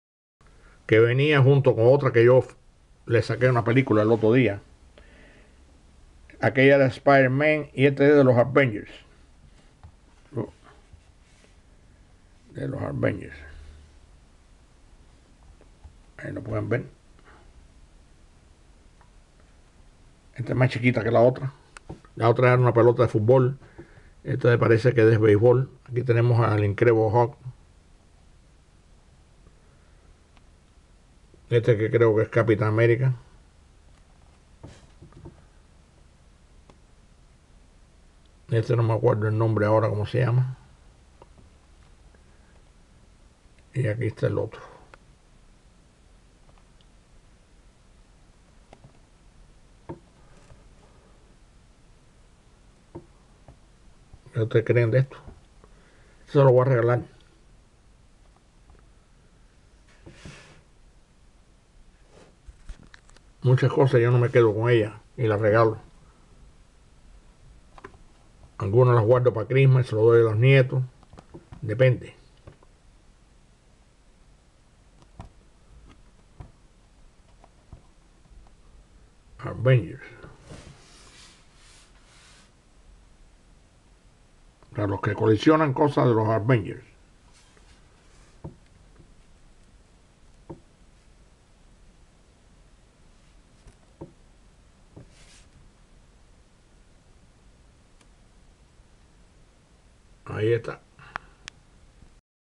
MARVEL AVENGERS BALL
SAID WHAT KIDS WANT,THIS MADE OF RUBBER,SAME SIDE THAN A BASEBALL.